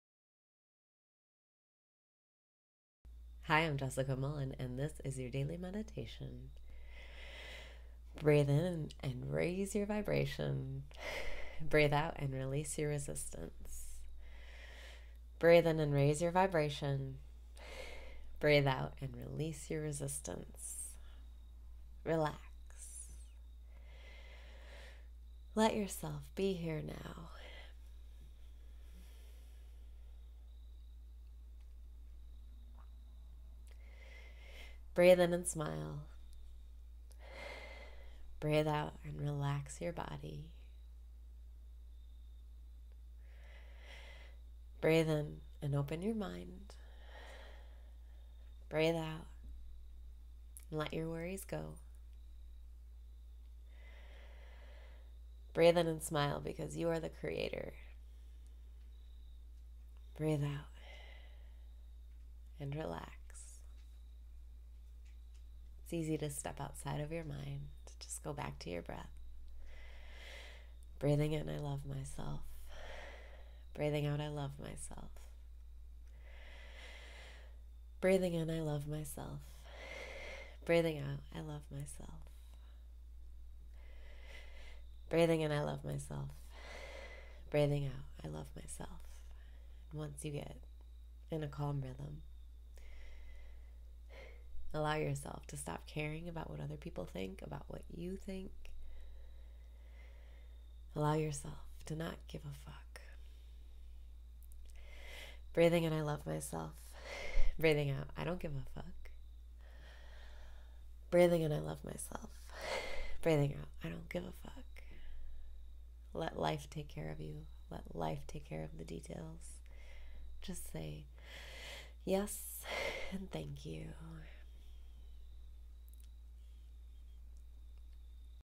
Daily Meditation - Enter a Calm Rhythm
Relax. Let yourself be here now.

Breathe in and open your mind!

Breathe in and smile because you are the creator!

It’s easy to step outside of your mind—just go back to your breath.

Once you get in a calm rhythm, allow yourself to stop caring about what other people think, about what you think, allow yourself to not give a fuck!

Let life take care of you. Let life take care of the details. Just say

YES!